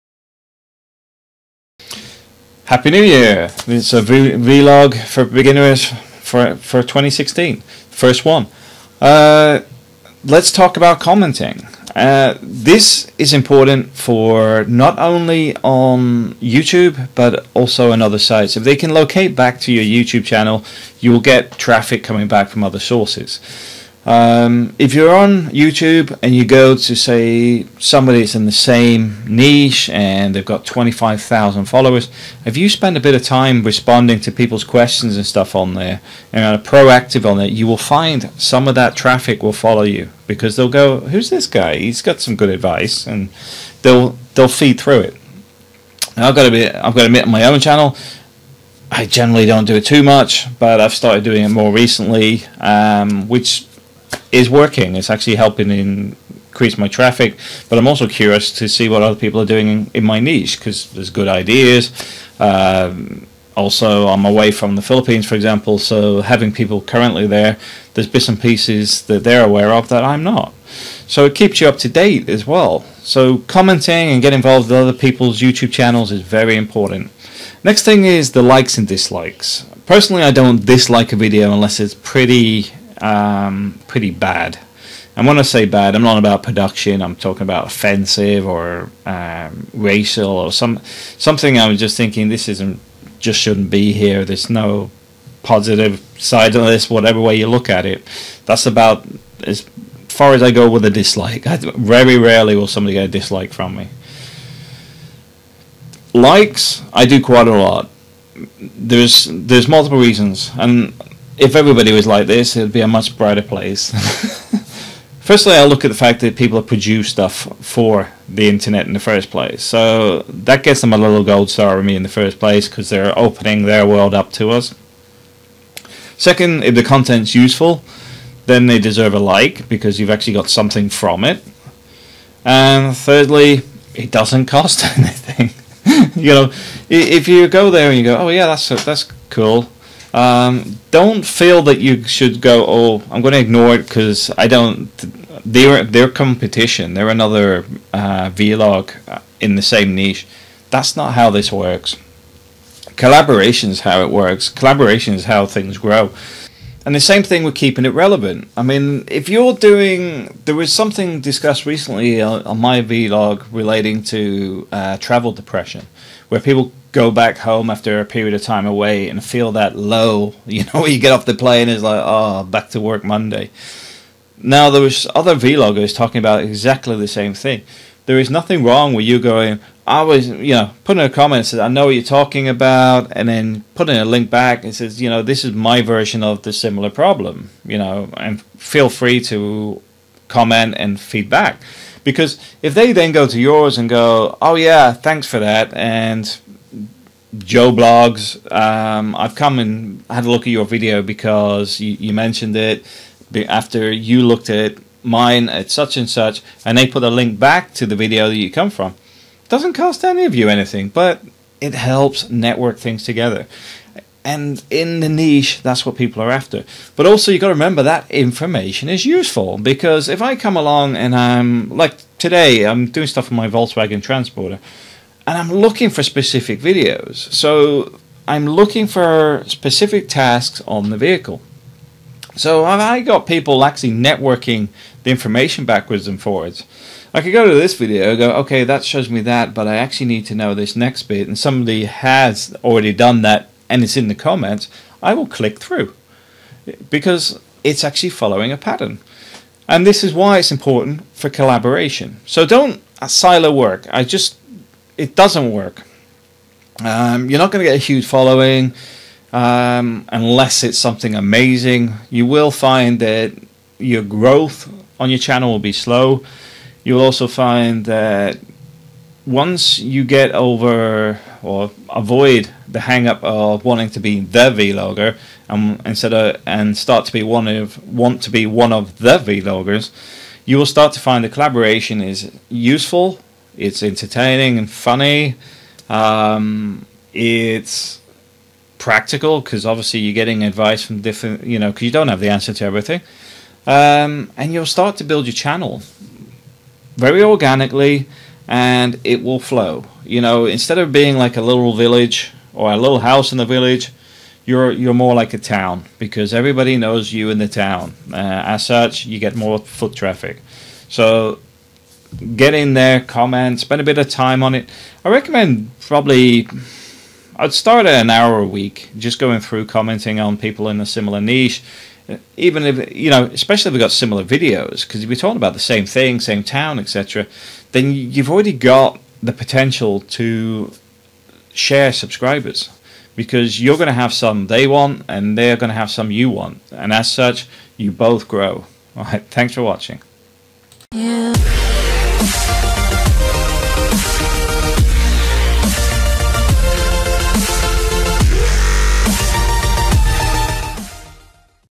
Vlogging For beginners Part 12: collaborate and comment
Vlogging for beginners this week discusses collaboration and commenting. As getting noticed online starts with networking with others, silo working doesn't help your channel but also you may find some useful information and tips talking with other Vloggers along the way.